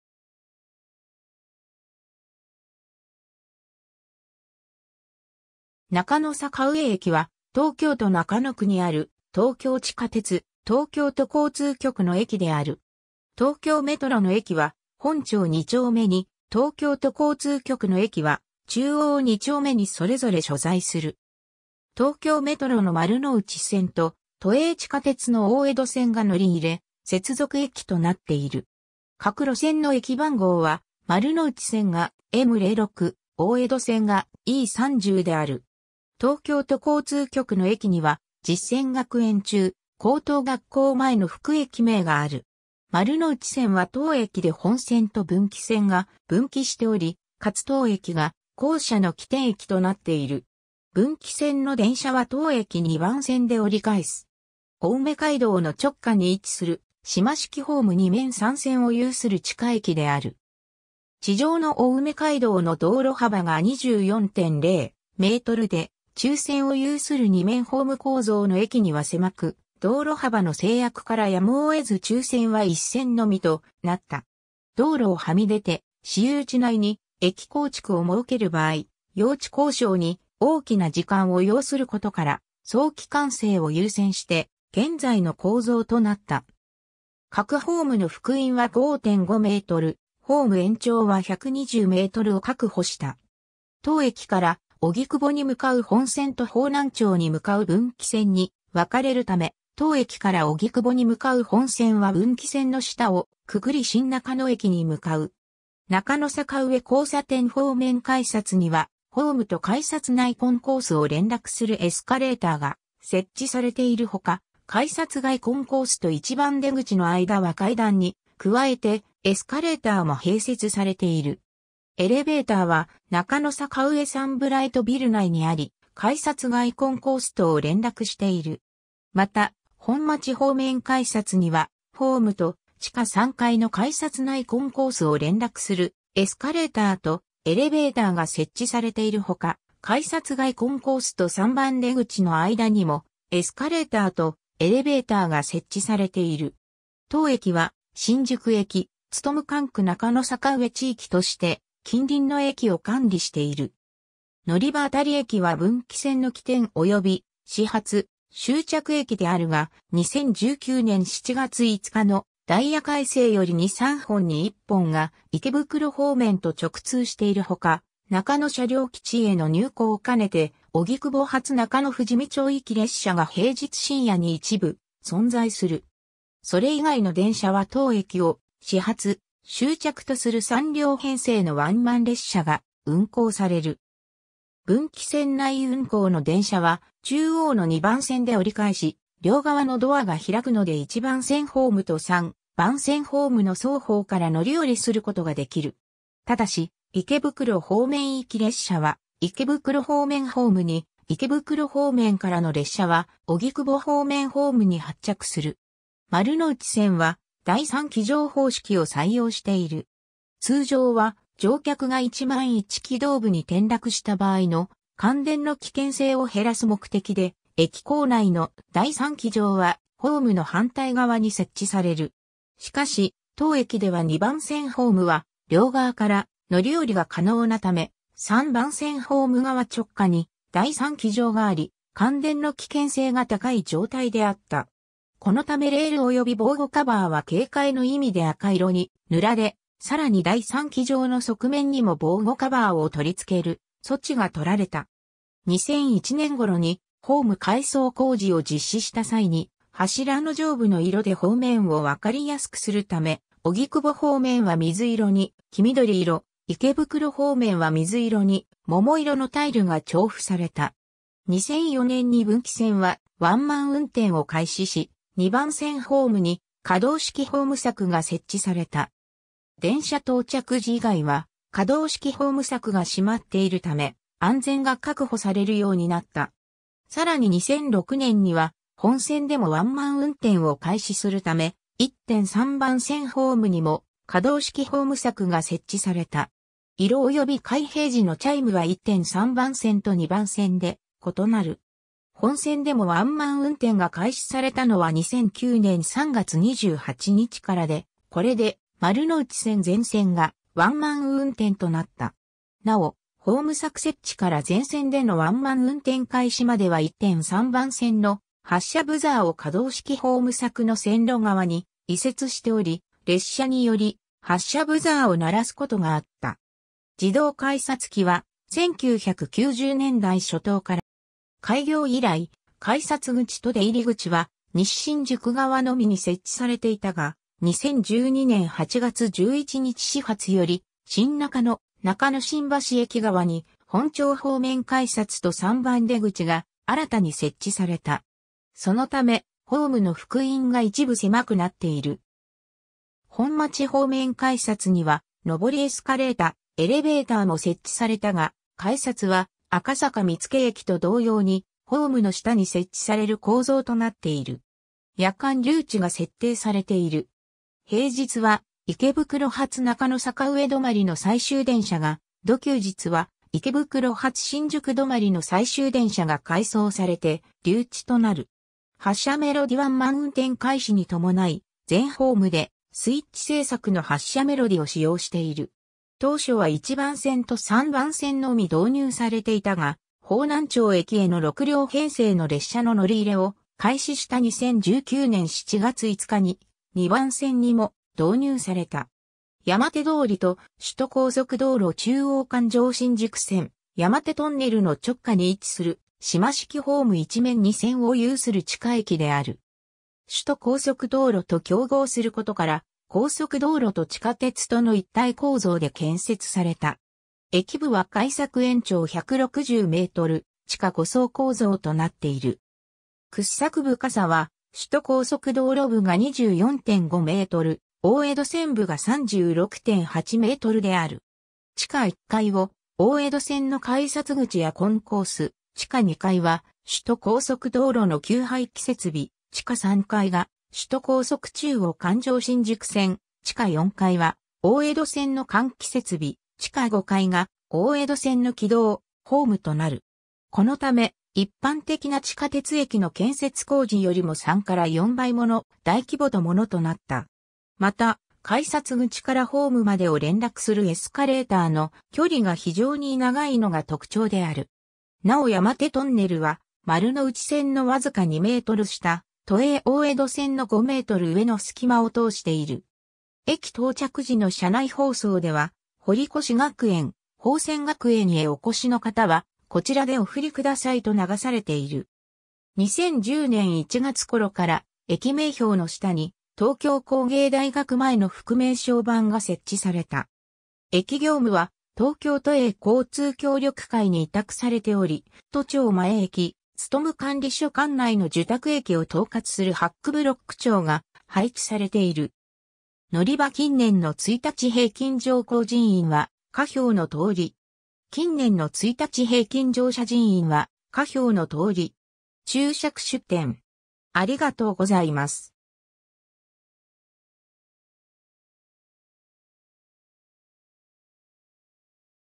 中野坂上駅
中野坂上駅（なかのさかうええき）は、東京都中野区にある、東京地下鉄（東京メトロ）・東京都交通局（都営地下鉄）の駅である。
 東京メトロの駅は本町二丁目に、東京都交通局の駅は中央二丁目にそれぞれ所在する。
 東京メトロの丸ノ内線と、都営地下鉄の大江戸線が乗り入れ、接続駅となっている。
各路線の駅番号は丸ノ内線がM 06、大江戸線がE 30である。
東京都交通局の駅には実践学園中・高等学校前の副駅名がある。
 丸ノ内線は当駅で本線と分岐線が分岐しており、かつ当駅が後者の起点駅となっている。
分岐線の電車は当駅2番線で折り返す。
 青梅街道の直下に位置する島式ホーム2面3線を有する地下駅である。
地上の青梅街道の道路幅が24.0 mで、中線を有する2面ホーム構造の駅には狭く、道路幅の制約から やむを得ず中線（分岐線）は1線のみとなった。
道路をはみ出て私有地内に駅構築を設ける場合、用地交渉に大きな時間を要することから、早期完成を優先して現在の構造となった。
各ホームの幅員は5.5 m、ホーム延長は120 mを確保した 。
当駅から、荻窪に向かう本線と方南町に向かう分岐線に分かれるため、当駅から荻窪に向かう本線は分岐線の下をくぐり新中野駅に向かう。
 中野坂上交差点方面改札には、ホームと改札内コンコースを連絡するエスカレーターが設置されているほか、改札外コンコースと1番出口の間は階段に加えてエスカレーターも併設されている。
エレベーターは中野坂上サンブライトビル内にあり、改札外コンコースとを連絡している。
また、本町方面改札にはホームと地下3階の改札内コンコースを連絡するエスカレーターとエレベーターが設置されているほか、改札外コンコースと3番出口の間にもエスカレーターとエレベーターが設置されている。
 当駅は、「新宿駅務管区中野坂上地域」として近隣の駅を管理している。
 のりば （出典：東京メトロ:構内図） 当駅は分岐線の起点および始発・終着駅であるが、2019年7月5日のダイヤ改正より2、3本に1本が池袋方面と直通しているほか、中野車両基地への入庫を兼ねて荻窪発中野富士見町行き列車が平日深夜に一部存在する。
それ以外の電車は当駅を始発・終着とする3両編成のワンマン列車が運行される。
 分岐線内運行の電車は中央の2番線で折り返し、両側のドアが開くので1番線ホームと3番線ホームの双方から乗り降りすることができる。
ただし、池袋方面行き列車は池袋方面ホーム（3番線）に、池袋方面からの列車は荻窪方面ホーム（1番線）に発着する。
 丸ノ内線は第三軌条方式を採用している。
通常は乗客が万一軌道部に転落した場合の感電の危険性を減らす目的で、駅構内の第三軌条はホームの反対側に設置される。
しかし、当駅では2番線ホーム（分岐線発着ホーム）は両側から乗り降りが可能なため、3番線ホーム側直下に第三軌条があり、感電の危険性が高い状態であった。
このためレールおよび防護カバーは警戒の意味で赤色に塗られ、さらに第三軌条の側面にも防護カバーを取り付ける措置がとられた。
 2001年頃にホーム改装工事を実施した際に、柱の上部の色で方面を分かりやすくするため、荻窪方面は水色に黄緑色、池袋方面は水色に桃色のタイルが貼付された。
 2004年に分岐線はワンマン運転を開始し、2番線ホームに可動式ホーム柵が設置された。
電車到着時以外は可動式ホーム柵が閉まっているため、安全が確保されるようになった。
さらに2006年には、本線でもワンマン運転を開始するため、1・3番線ホームにも可動式ホーム柵が設置された。
色および開閉時のチャイムは1・3番線と2番線で異なる。
本線でもワンマン運転が開始されたのは2009年3月28日からで、これで丸ノ内線全線がワンマン運転となった。
なお、ホーム柵設置から全線でのワンマン運転開始までは1・3番線の発車ブザーを可動式ホーム柵の線路側に移設しており、列車により発車ブザーを鳴らすことがあった。
 自動改札機は、1990年代初頭から各駅への本格導入が始まったが、それ以前にも、1974年頃から有楽町線銀座一丁目駅・池袋駅、東京メトロ日比谷線恵比寿駅とともに試験的に設置されていた。
 開業以来、改札口と出入口は西新宿側のみに設置されていたが、2012年8月11日始発より、新中野・中野新橋駅側に本町方面改札と3番出口が新たに設置された。
そのため、ホームの幅員が一部狭くなっている。
本町方面改...